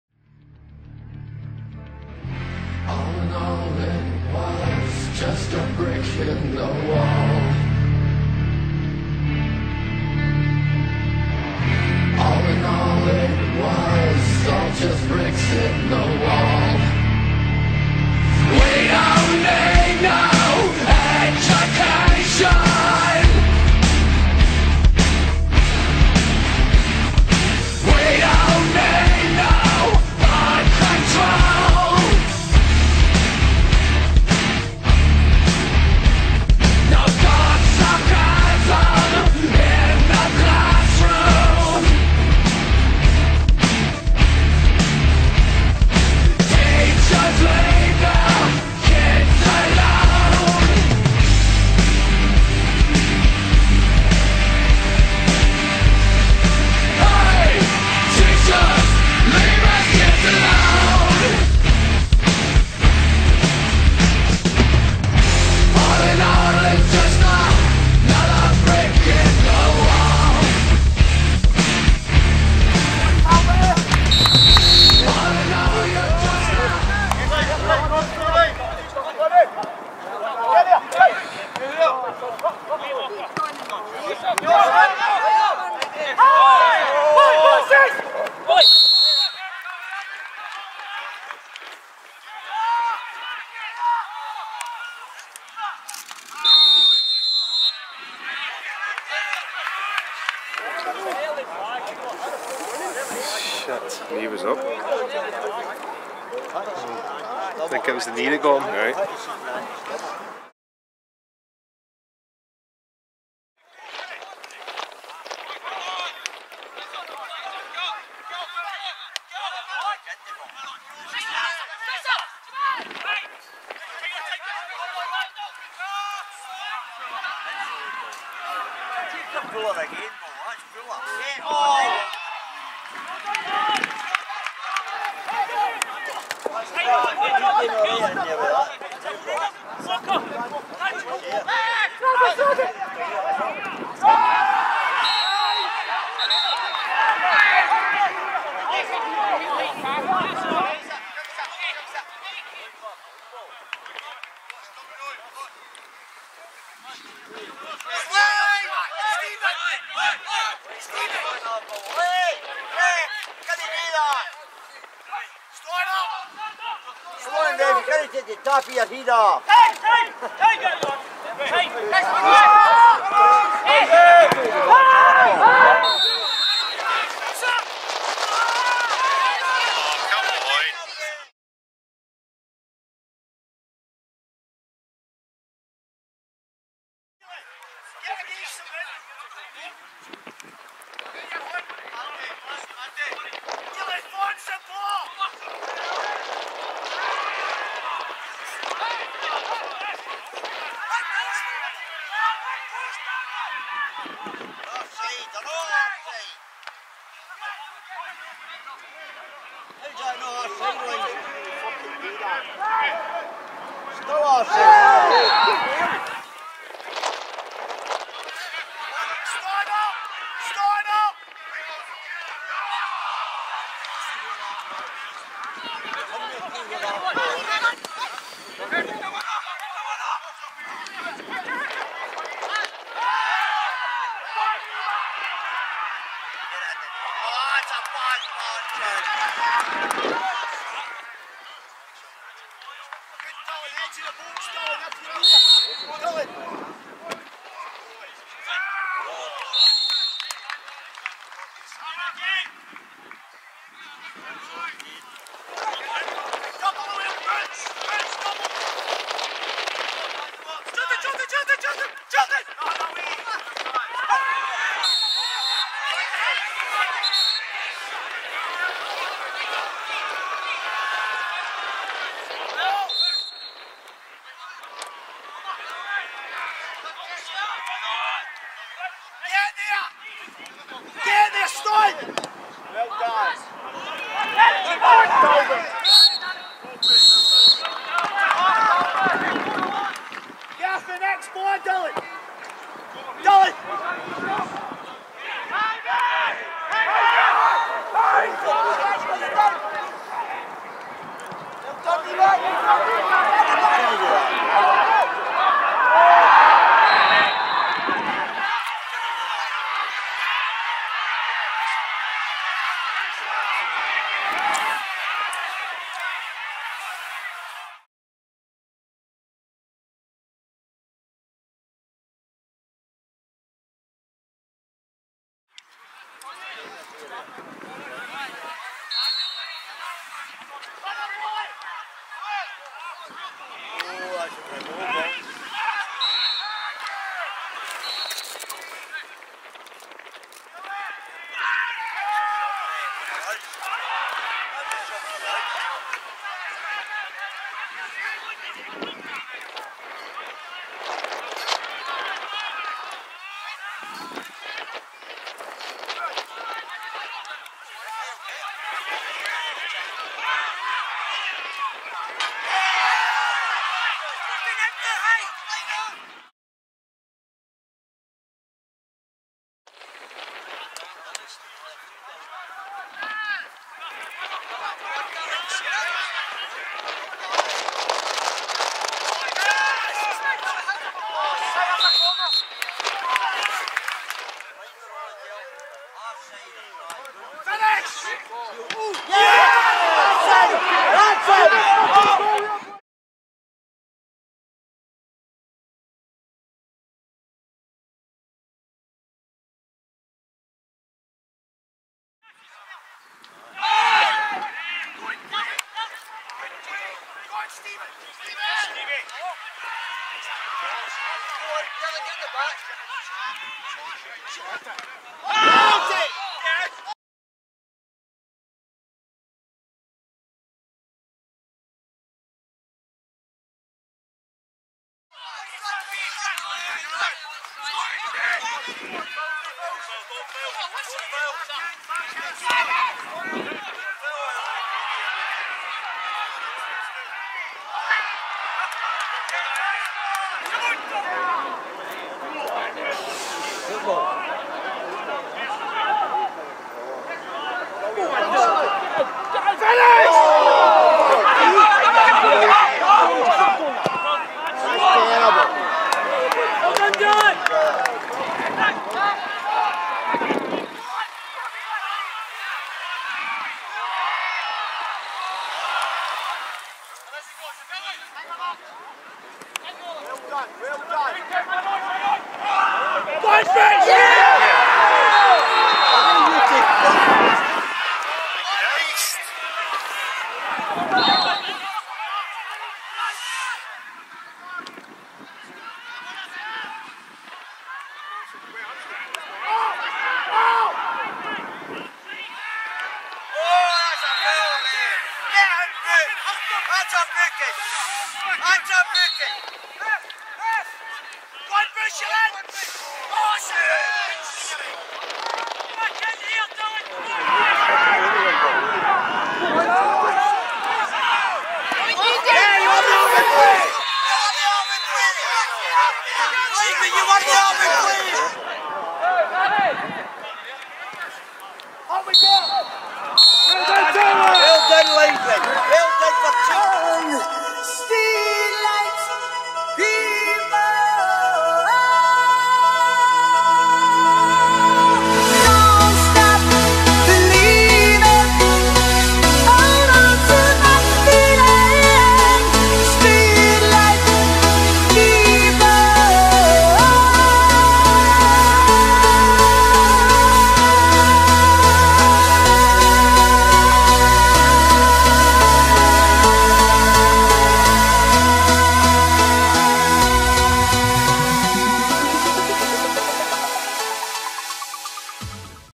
Match Highlights: Kirkintilloch Rob Roy 0-0 Auchinleck Talbot
The chase for the title remains wide open after both sides had a share of the points.